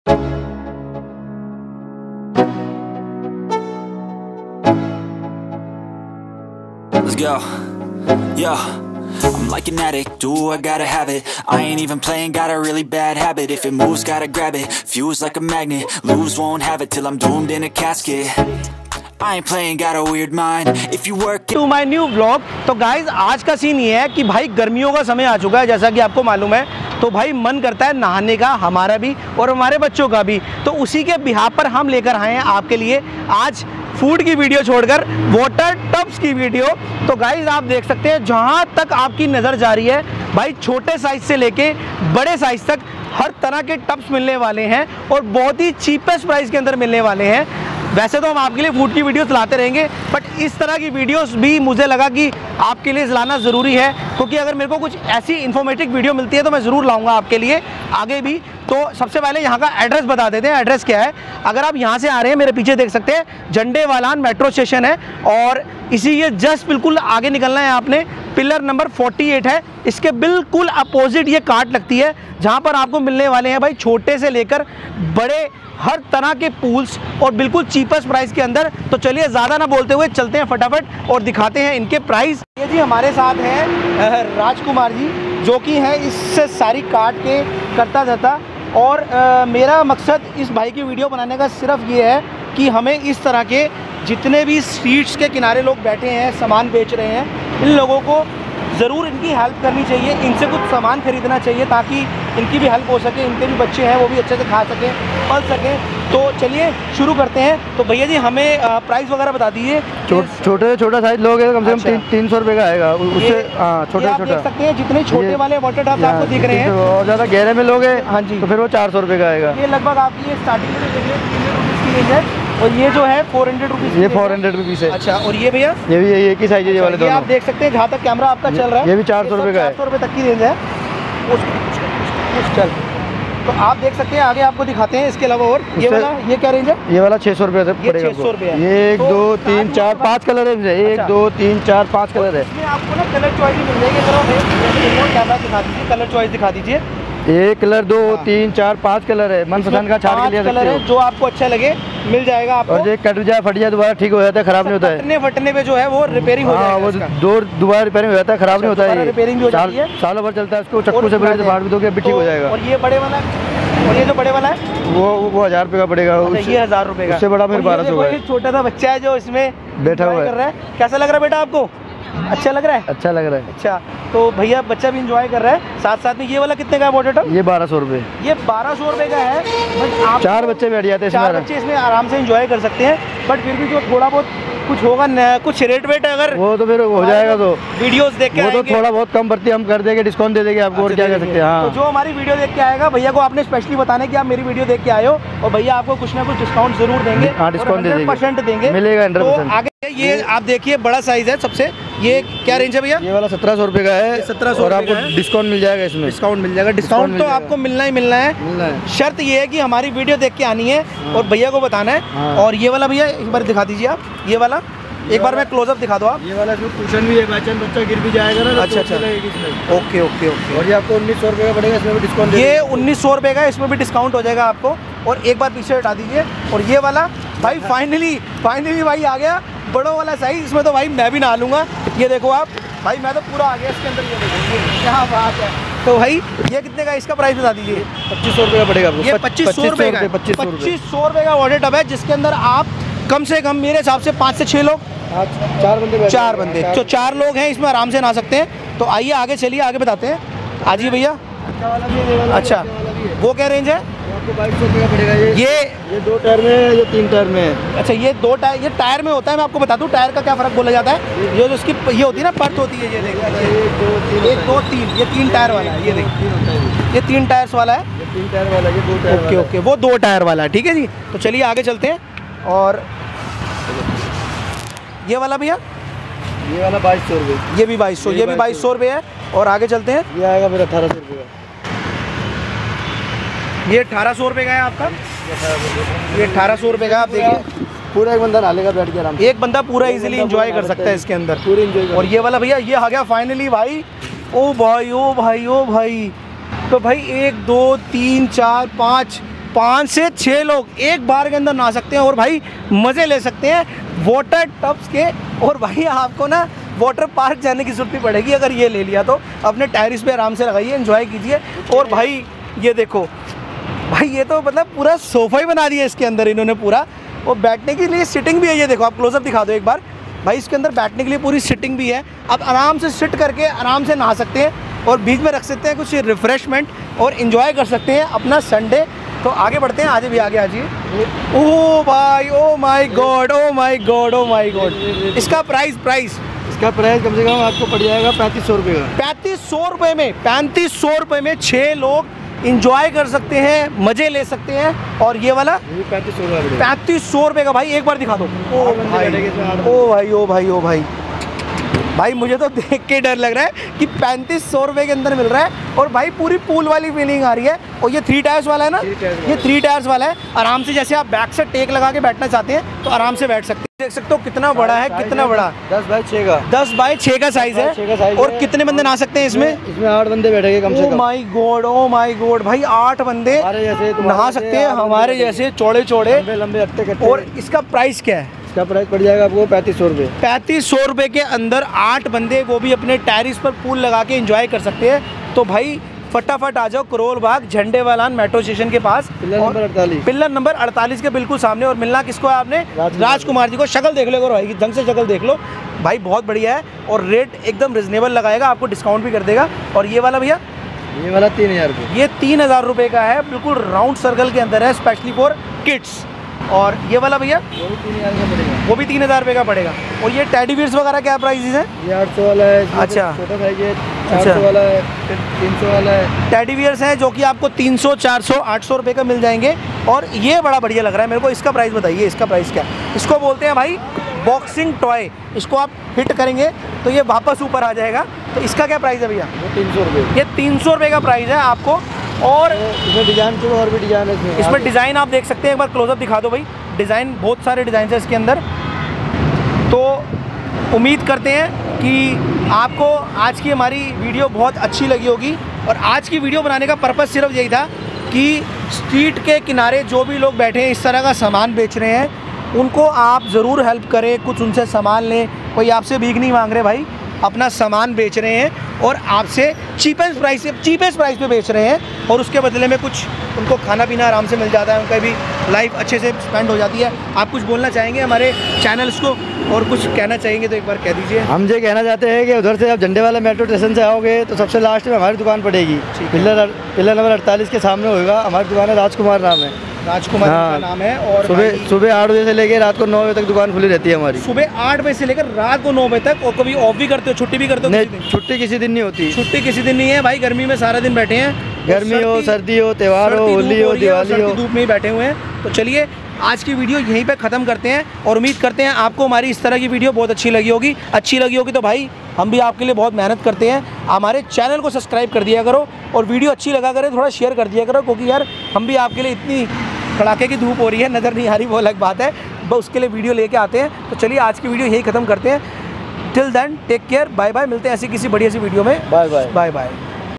Cheapest Water Tubes In Delhi Karol Bagh Staring Only 200 Rs
Hello Foodies Wellcome To Our Channel
Today We Cover Water Tub

Shop Adress = Karol Bagh Near Jhandewalan Metero Station Opposite Piller No 48

Price List In Video

Opinng Hours :8.00Am To 10 Pm